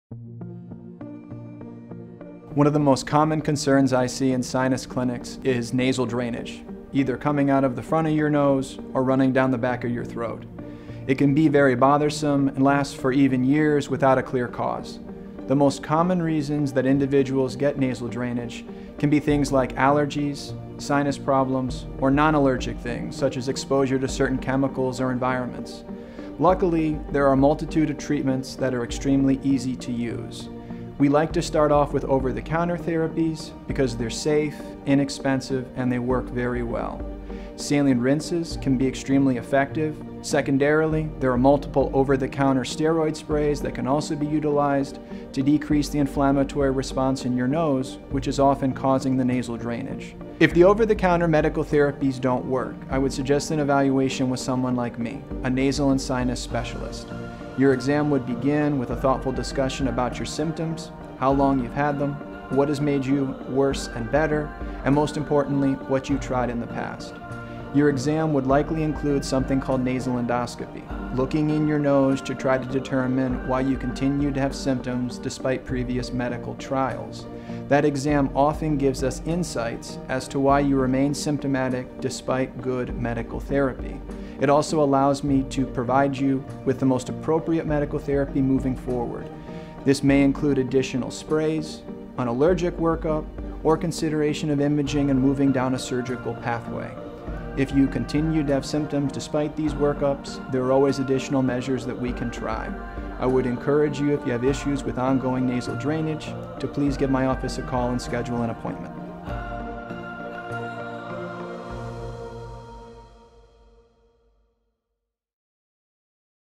Solving Nasal Drainage Issues
Whether it’s running down your nose or the back of your throat, Kenneth Rodriguez, MD, Chief of Rhinology and Skull Base Surgery at University Hospitals Ear, Nose and Throat Institute, identifies the causes, shares easy to use therapies and provides a look into more extensive exams and treatments to solve this common sinus issue.